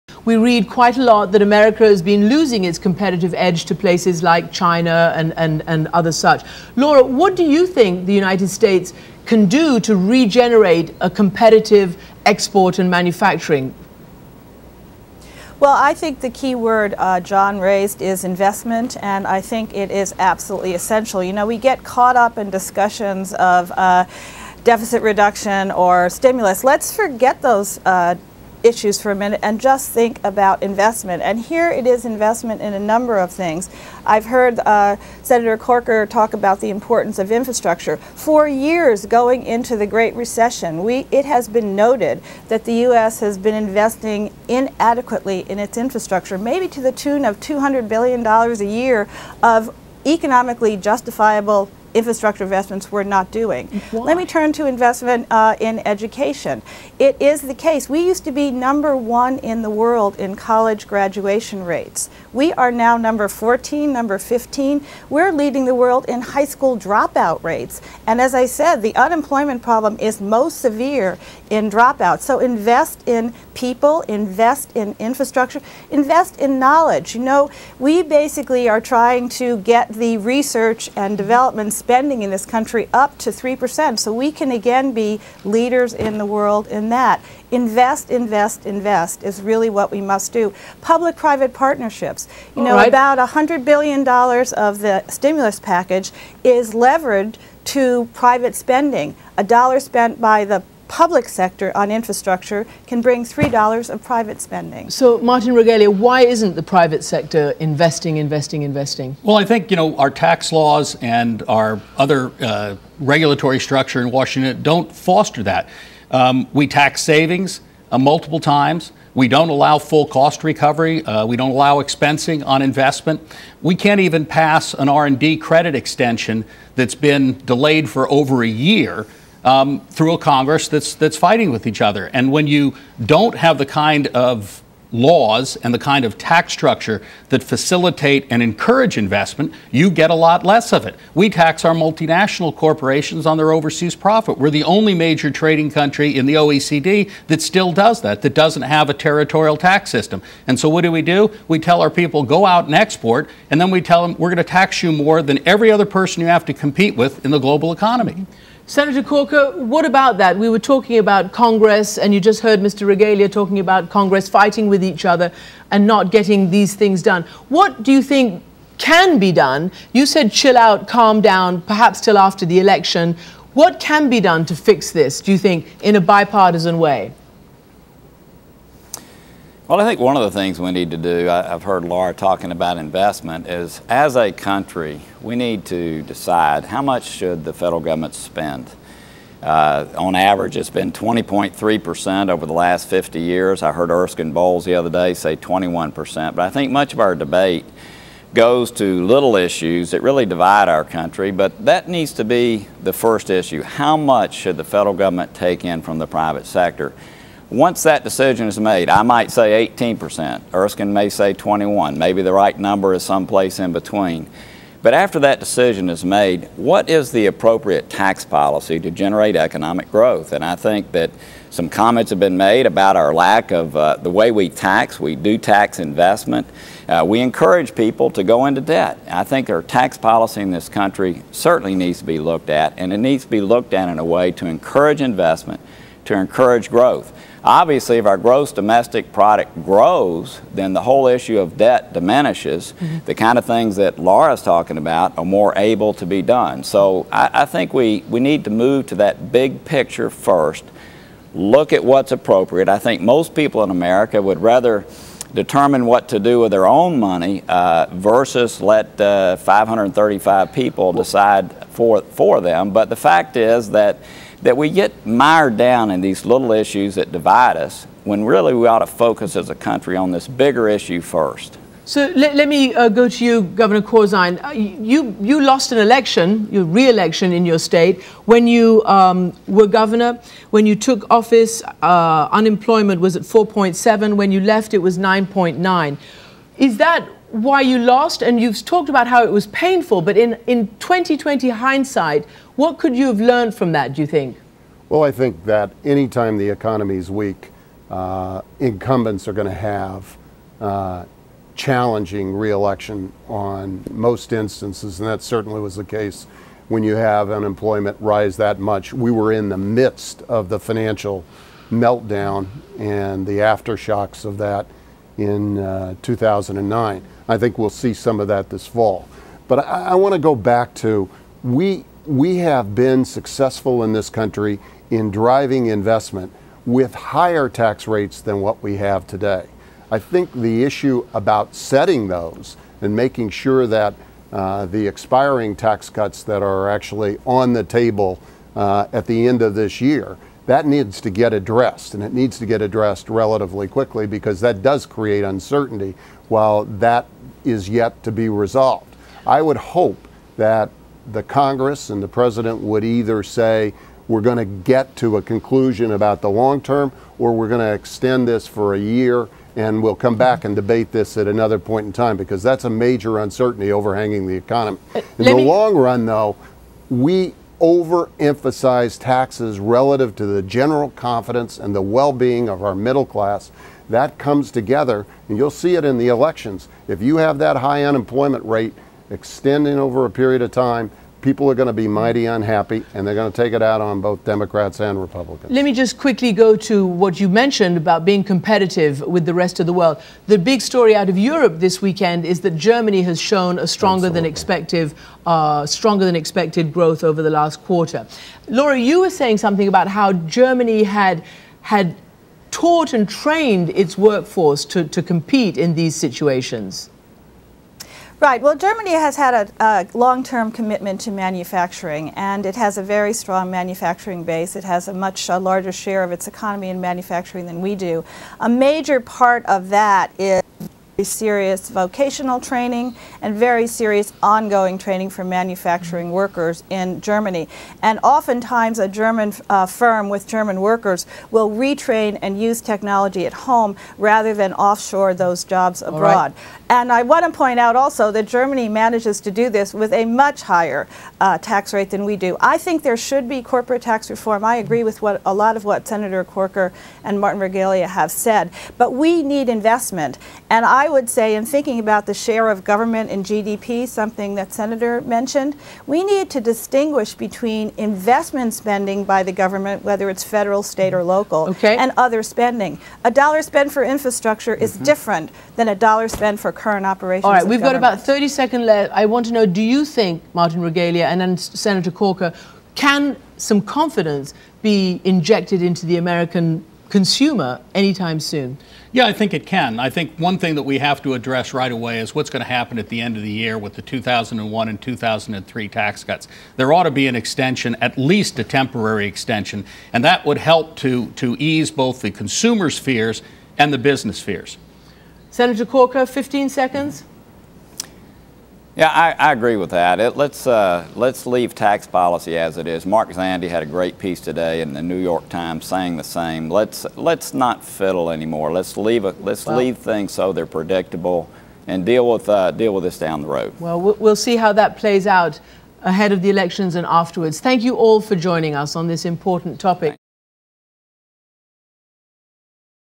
Experts Examine the Economy (Part 2)
Bob Corker, Jon Corzine, Laura Tyson and Martin Regalia discuss the economy.